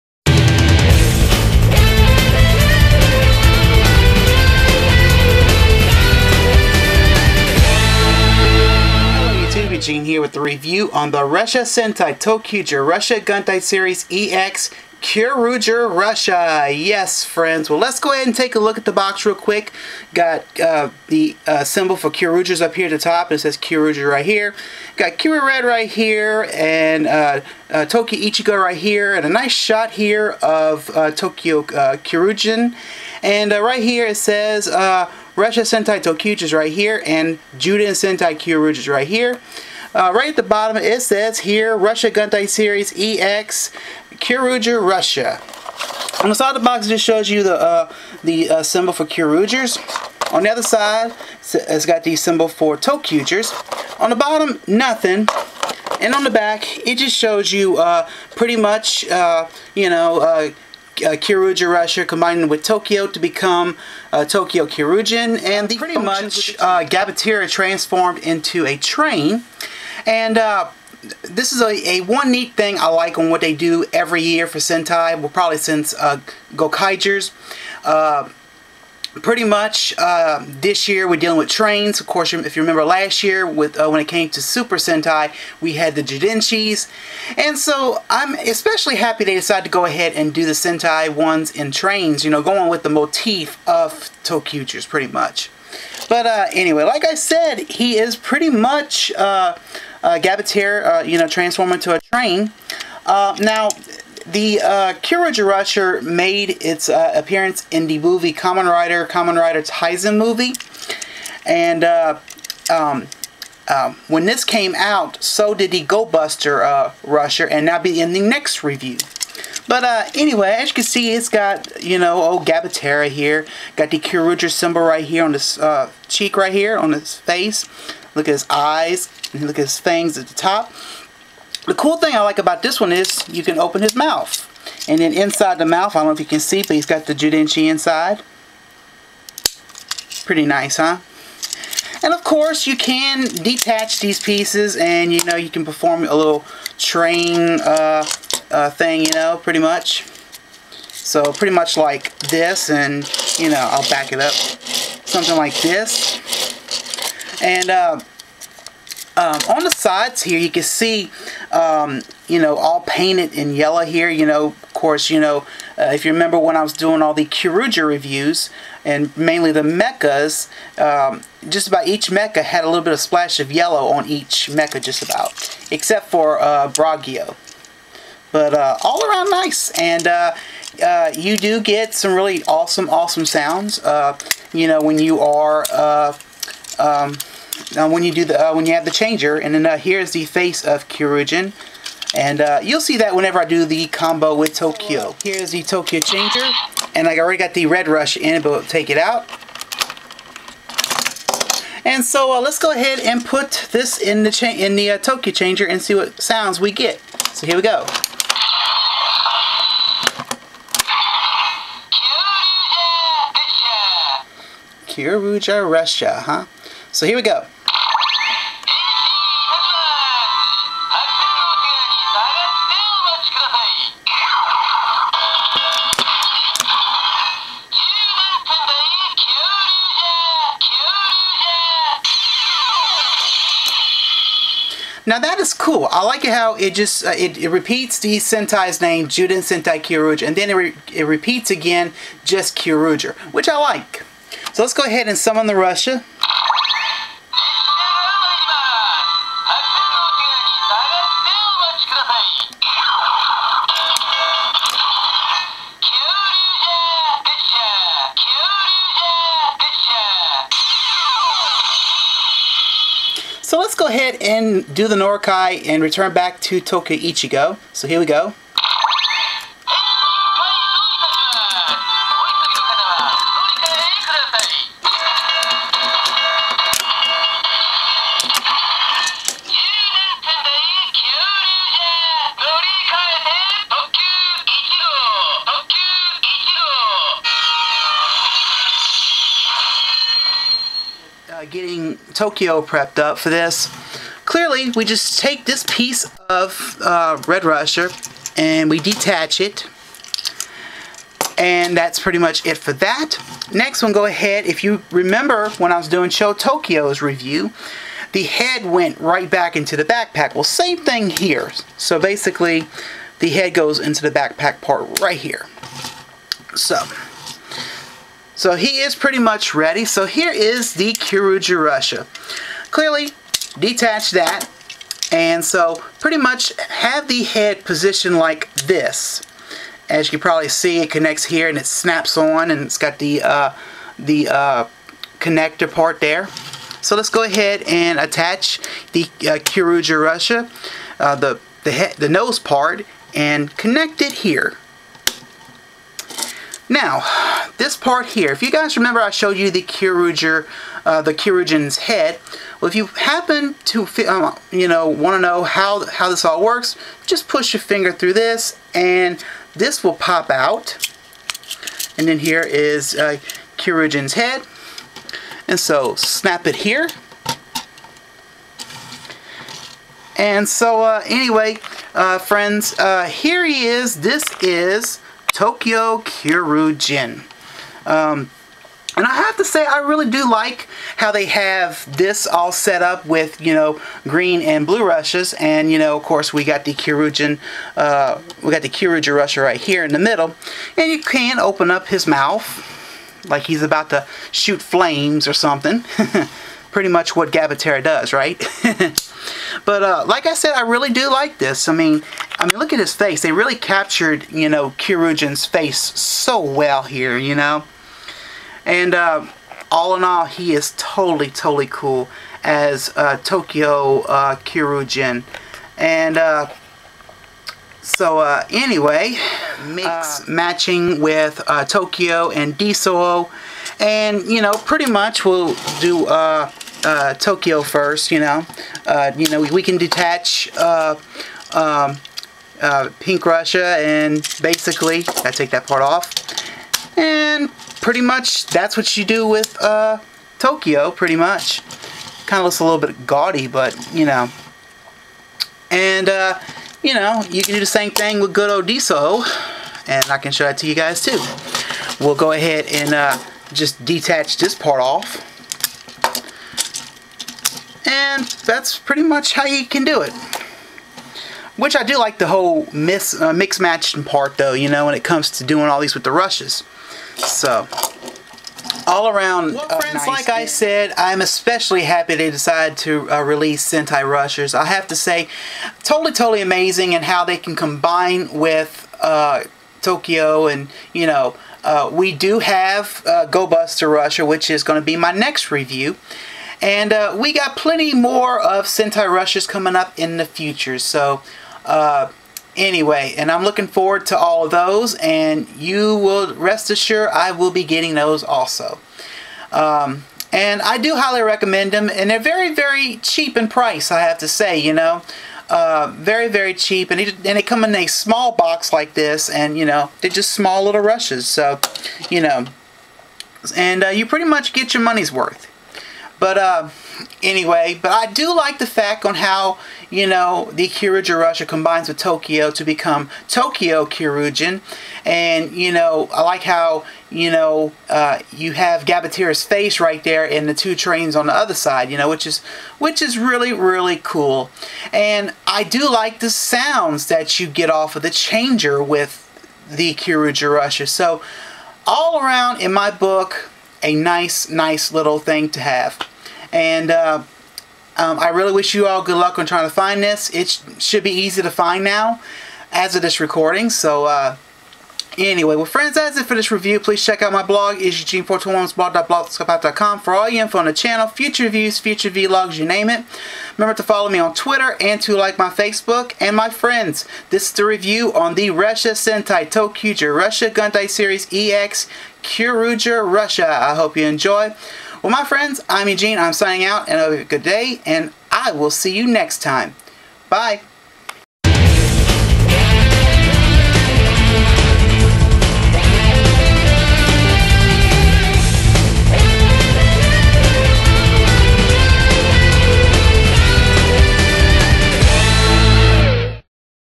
Review: Ressha Sentai ToQger - Ressha Gattai Series EX Kyoryuger Ressha
Hi Friends,

Welcome to a Review on the Ressha Sentai ToQger - Ressha Gattai Series EX Kyoryuger Ressha.    The Kyoryuger Ressha made it's appearance in the Movie Kamen Rider Kamen Rider Taizen Movie.    The Sounds that comes off the Changer are amazing does the name of the Sentai (Zyuden Sentai Kyoryuger, Kyoryuger).      The Kyoryuger Ressha can combine with ToQ-Oh to become ToQ-Oh Kyoryuzin and making it a Nice Combo with Trains and Dinos.  I love the Sentai Ressha and looking forward to more.

-Eugene